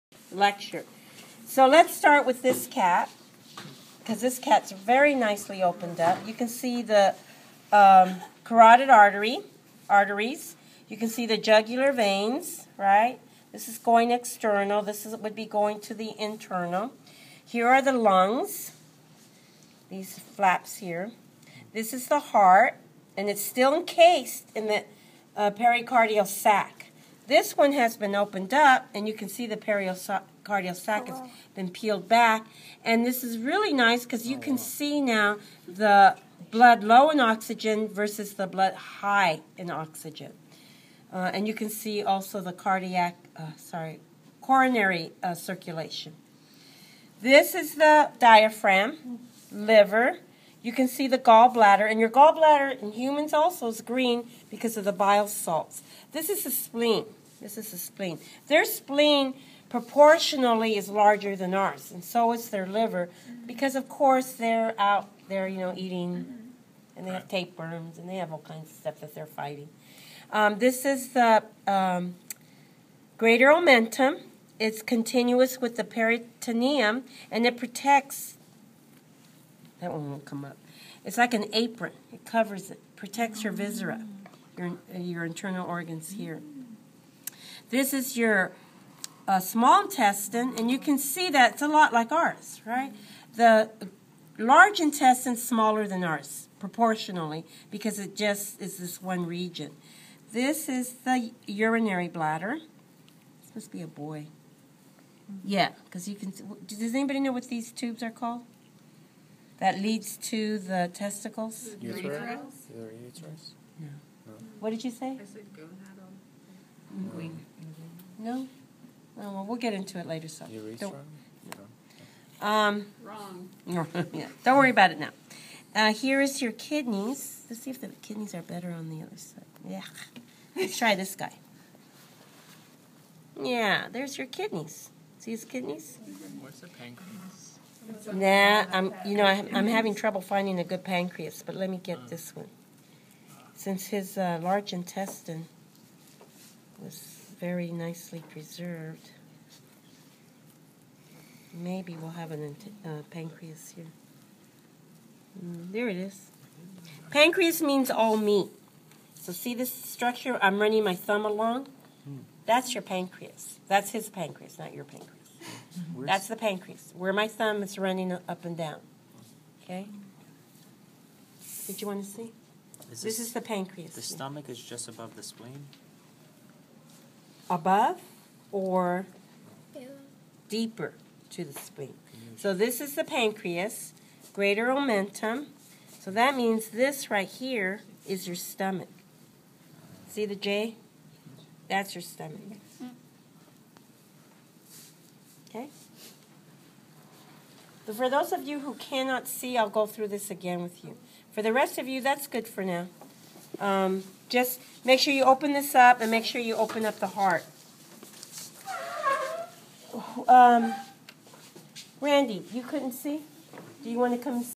Digestive system of cat
This is just a overviews of Digestive system of a cat (female and male) for introductory to Anatomy 1. Hope its helpful, and I apologize for any bad quality of the picture, since my hand was tired of holding the camera (there are small shakes during the video). Enjoy. :)